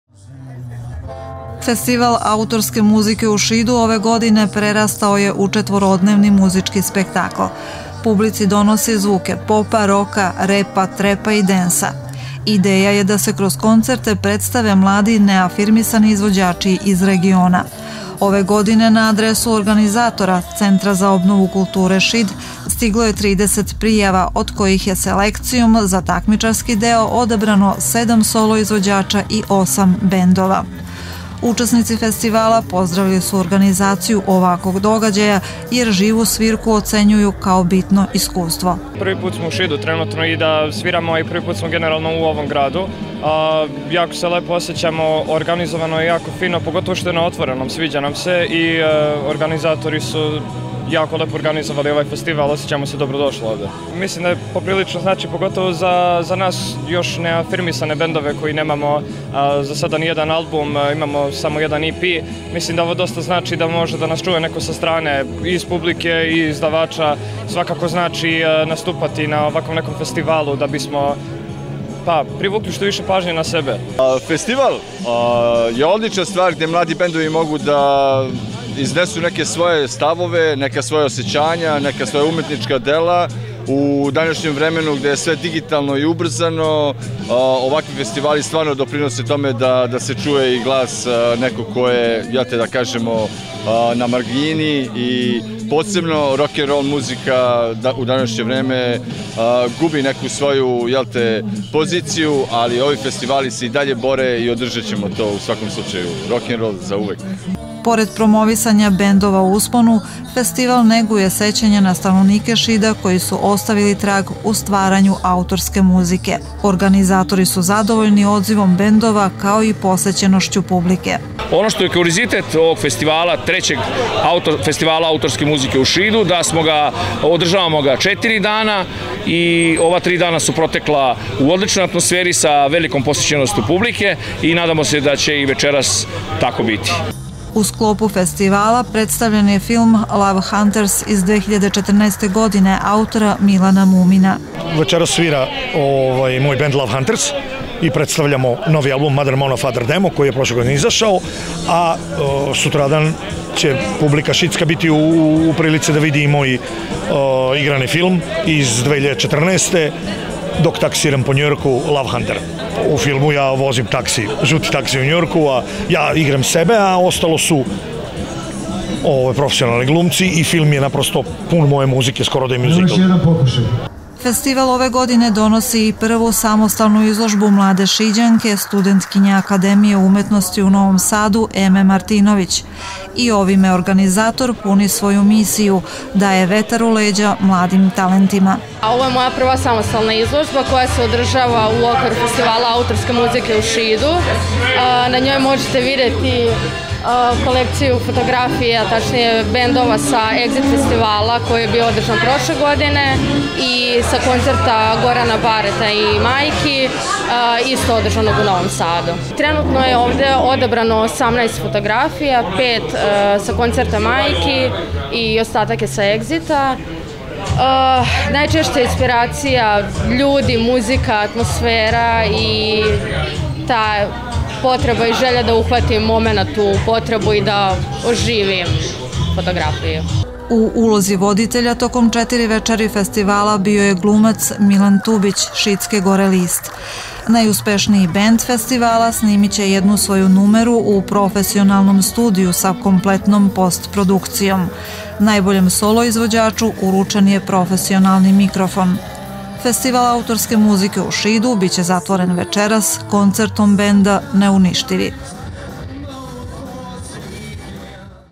3. Festival autorske muzike
Treći po redu Festival  autorske muzike otvoren je u petak, 16. avgusta na platou ipred kafea BarOK u Šidu.   Festival je takmičarskog karaktera:  prve večeri predstavili su se mladi solisti dok su   druge večeri nastupili bendovi. U sklopu Festivala   otvorena je  izložba fotografija mlade šiđanke  Eme Martinović. Festival traje četiri dana i biće zatvoren koncertom grupe Neuništivi